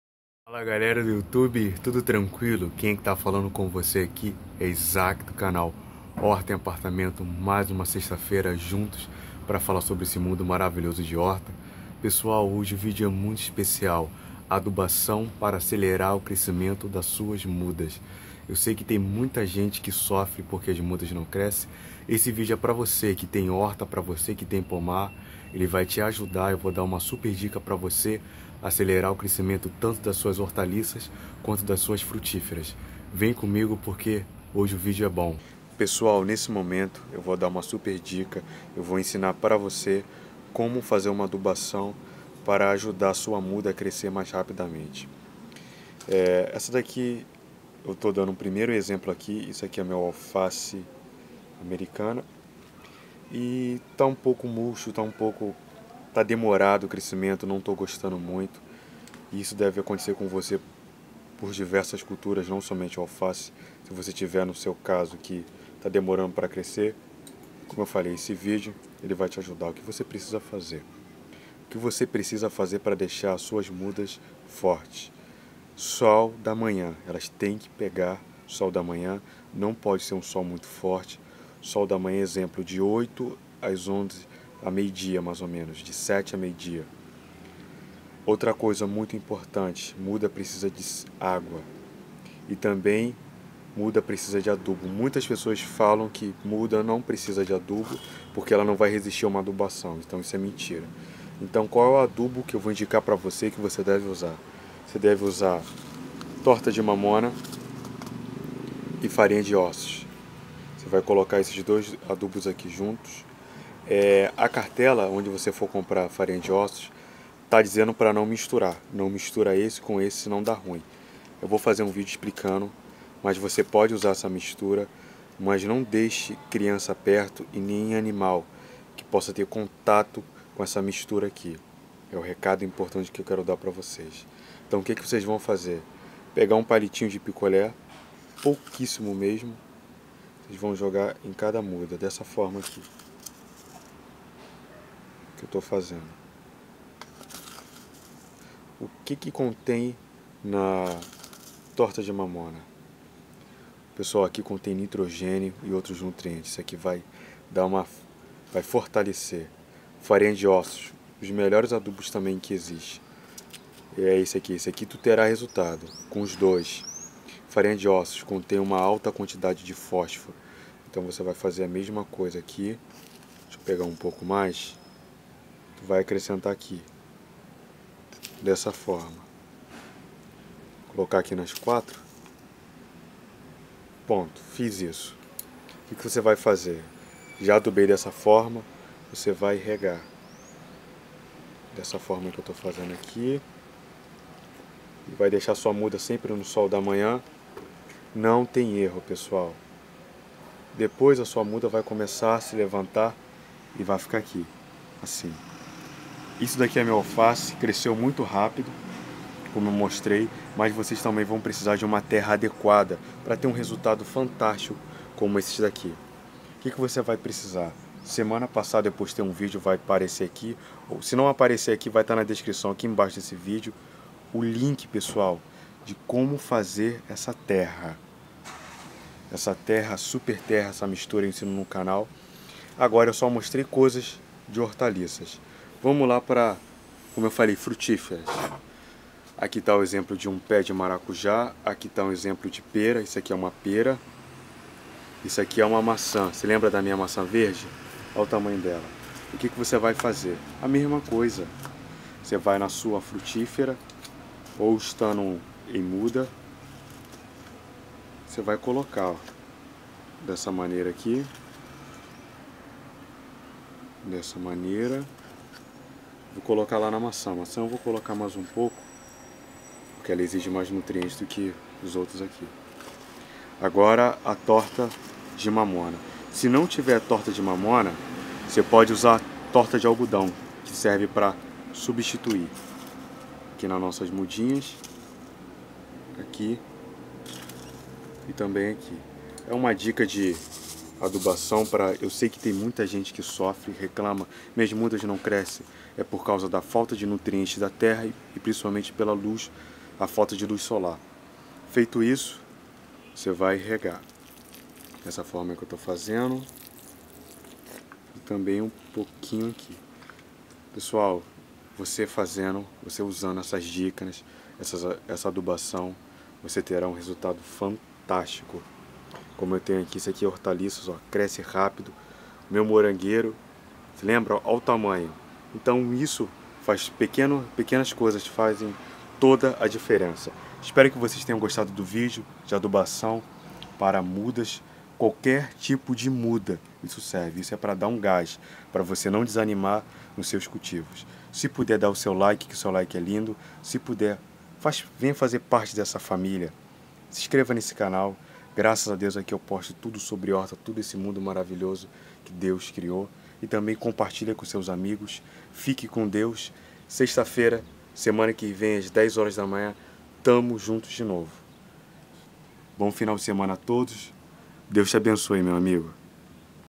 Super Adubação para Mudas(Crescimento ate 3x mais Rápido)
Nesse vídeo ensino a fazer um ótimo adubo para mudas que estão com problemas de crescimento tudo muito simples e fácil.

Marias para o adubo . Farinha de ossos
                                          Torta de Mamona .

Como Fazer . pegue esses dois elementos  coloque um pouco em cada muda depois molhe . ai suas mudas ficaram fortes novamente
Um dica não se esqueça do sol nas mudas .